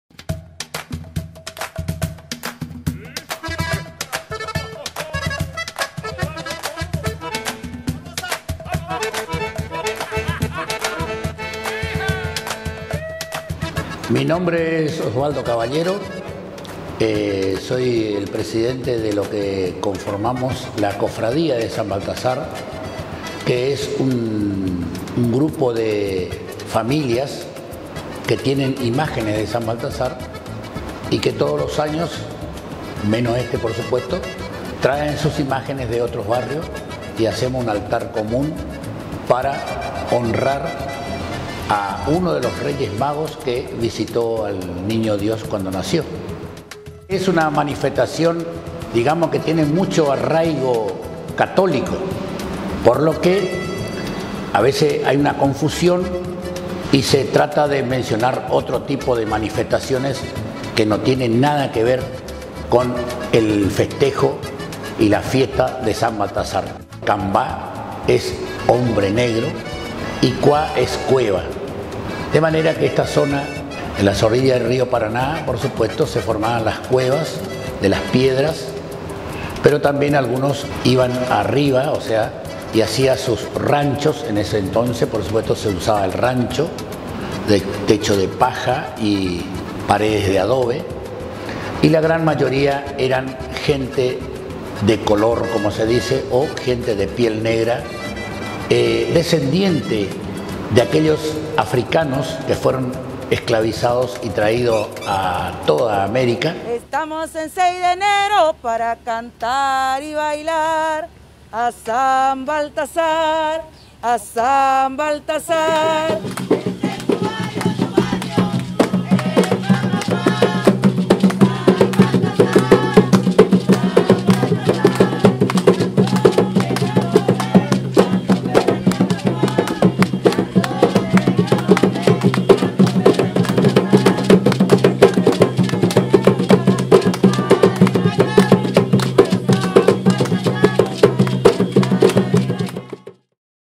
San Baltasar 2021
Corrientes, conocida por su historia, religión y cultura, se vistió de rojo, amarillo y blanco para recibir la llegada del santo mas candombero de todos, el santo de la alegría, el santo cambá 🤴🏿🥁❤
Todos los 6 de enero -en las calles del barrio CambáCua- mediante actos religiosos, ofrendas y bailando danzas de influencias africanas, rendimos culto a San Baltasar, uno de los tres reyes magos de la epifanía cristiana.
Venerado en toda América, este santo comenzó a ser homenajeado en nuestra ciudad a partir del 1700, puntualmente en el barrio Cambá Cua (que en guaraní significa “cueva de negros”), ya que allí se encontraban asentados los esclavos afrodescendientes.
¿Alguna vez participaste en las celebraciones de San Baltasar? Este año, debido a la situación sanitaria que atravesamos, no pudimos celebrarlo como acostumbramos, pero la imagen del Santo recorrió su amado barrio y llevó bendiciones y alegría a todas las familias de la zona ♥️
¿Querés conocer más sobre los atractivos turísticos de la ciudad? Ingresá a nuestra web